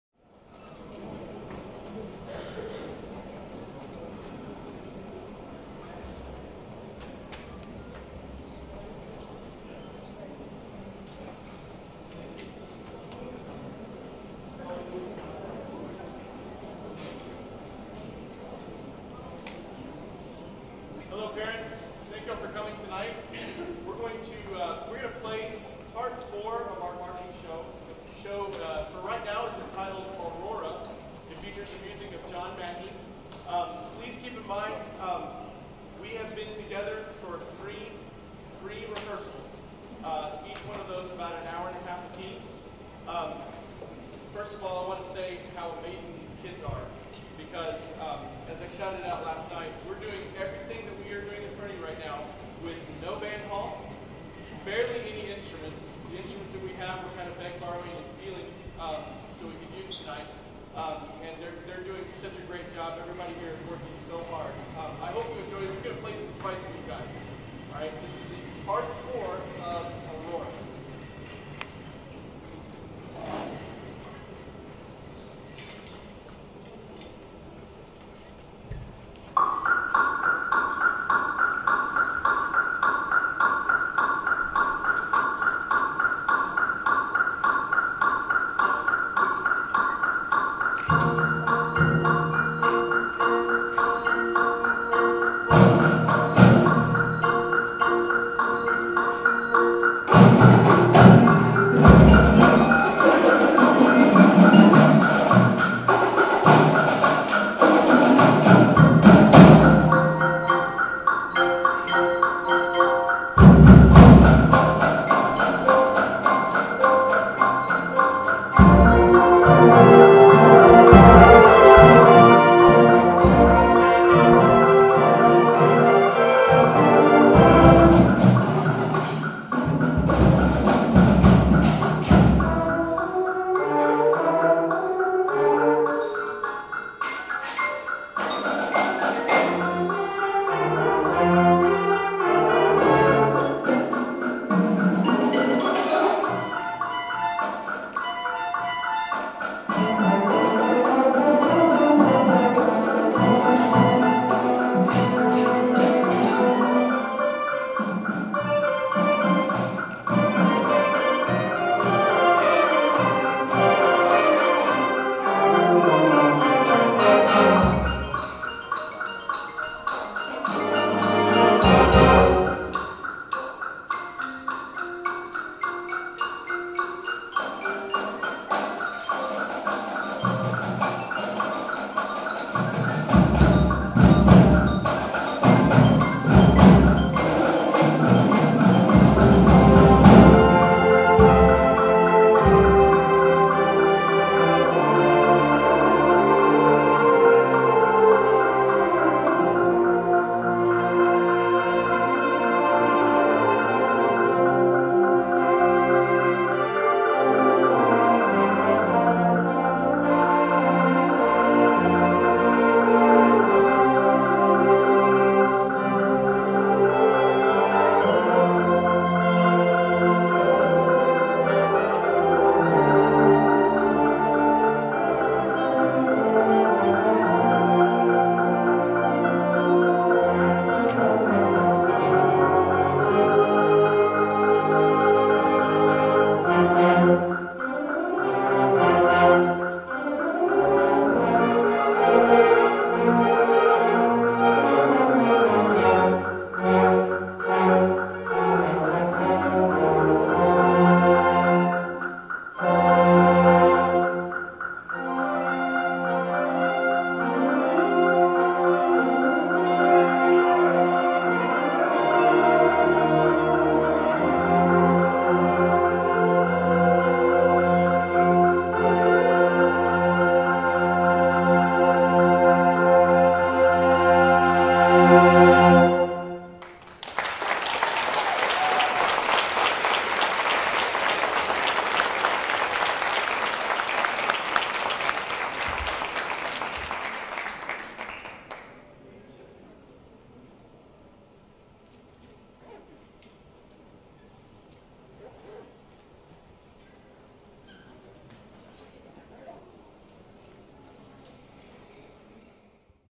Grand Oaks High School Band
Hear our first growl.  First performance of the Grand Oaks Grizzly Band